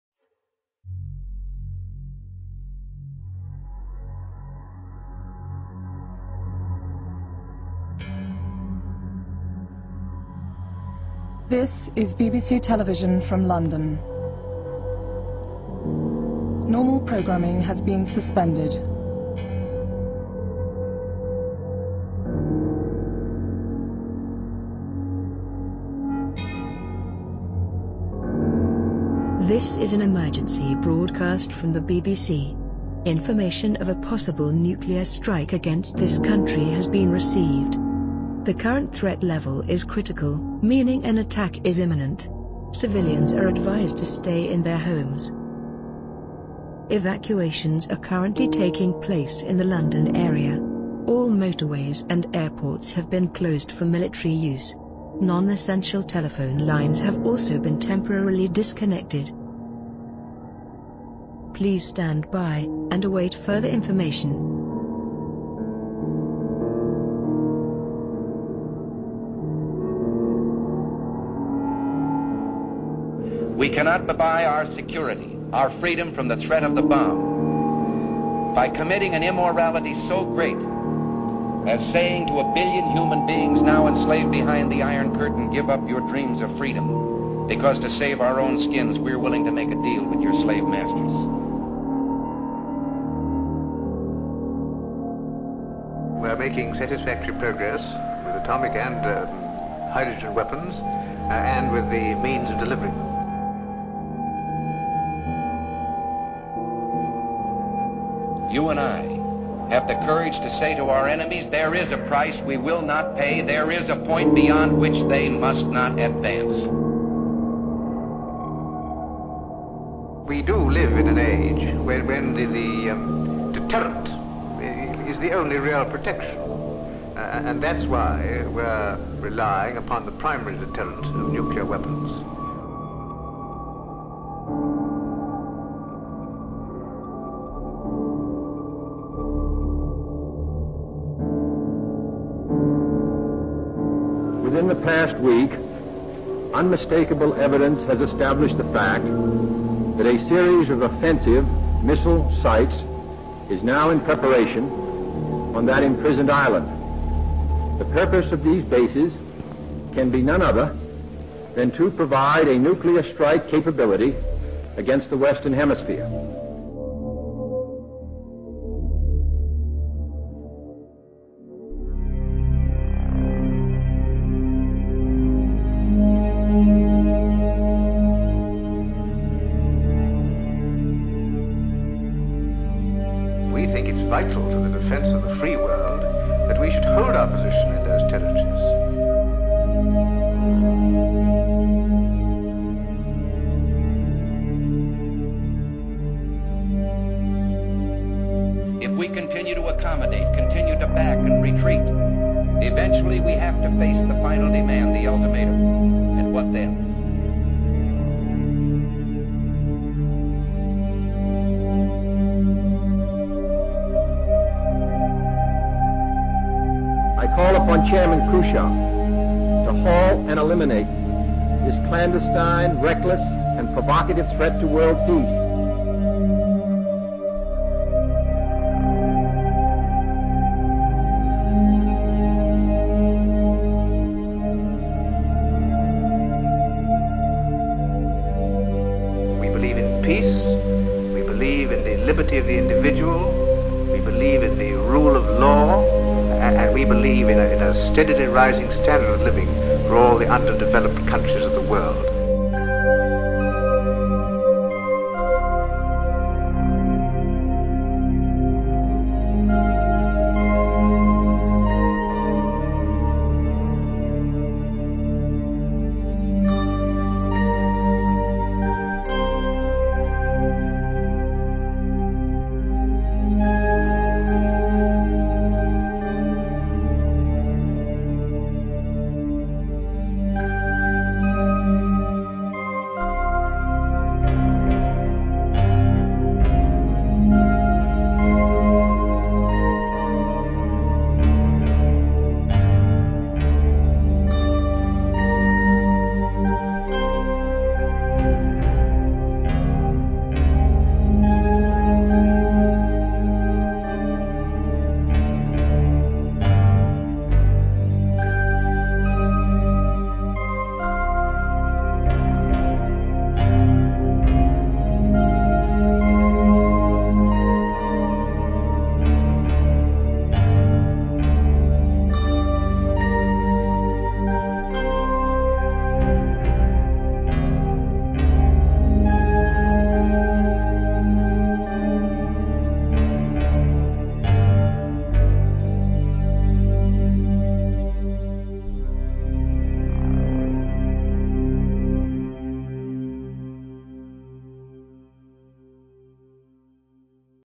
Exploring The Secret Nuclear Bunker - Kelvedon Hatch.
A look inside the secret nuclear bunker at Kelvedon Hatch, Essex. A nuclear bunker that was built in 1952/53 as part of Rotor.  This was a program to improve an harden Britain's air defense network.  It was constructed by Peter Lind and Co. of London who still trade today.  The bunker was hardened (three level 'R4') Sector Operations Centre (SOC) for RAF fighter command and was 'live' until 1994.